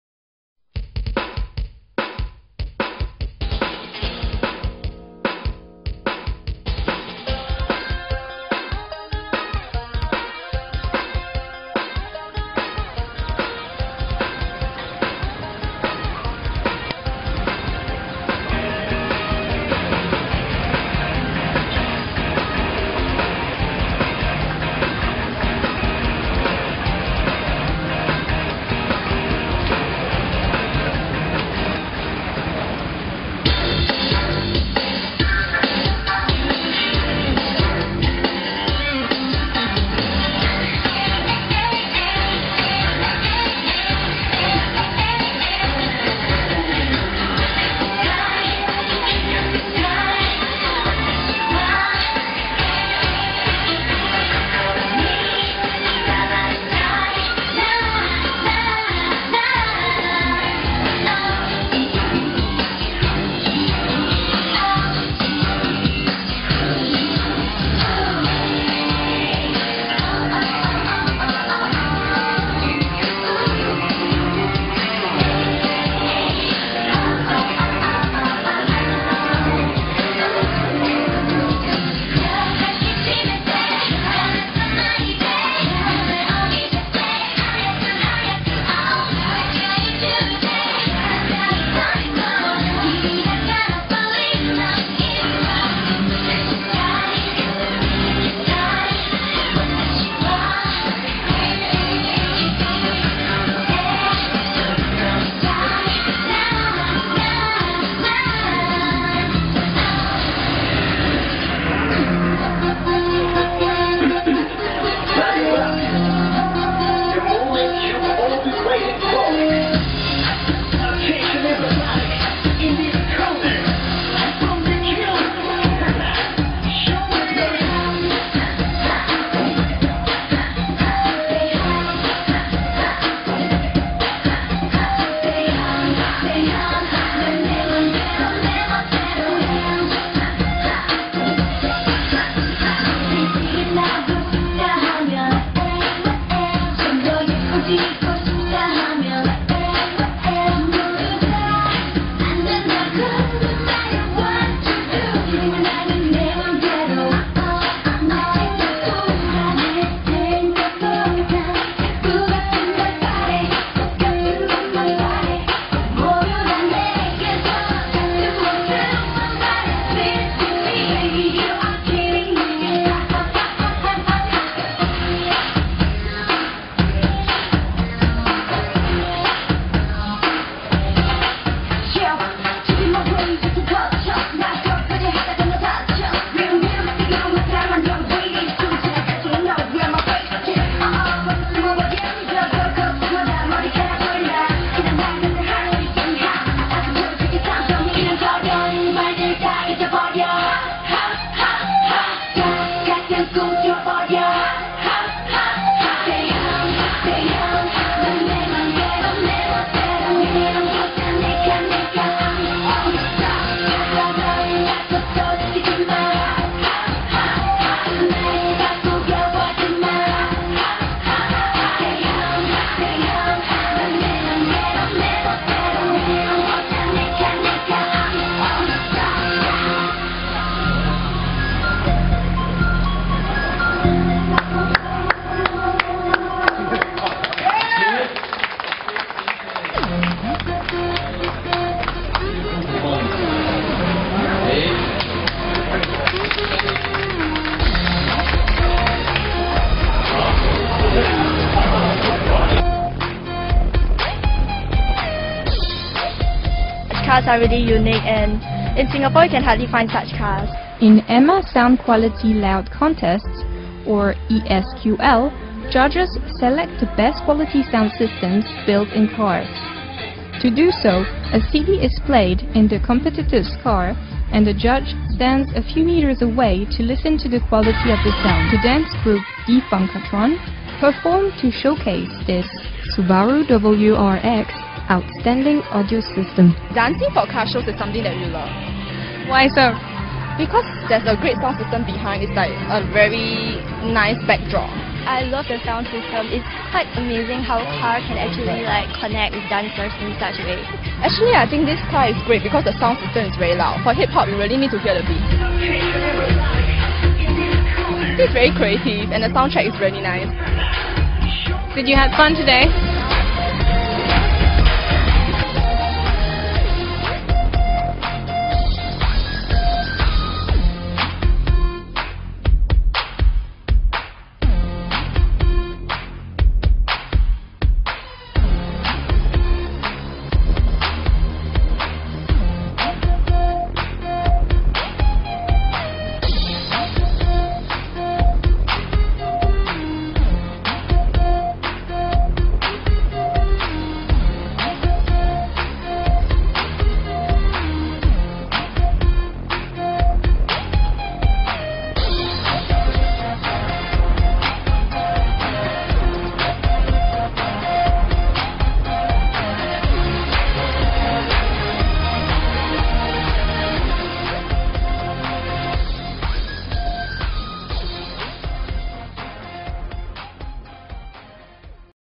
3 of DFunkatron girls performing @ Emma Singapore ESQL 2011 (Pioneer Show Car) Subaru WRX.mp4
by DANCE FACTORY SINGAPORE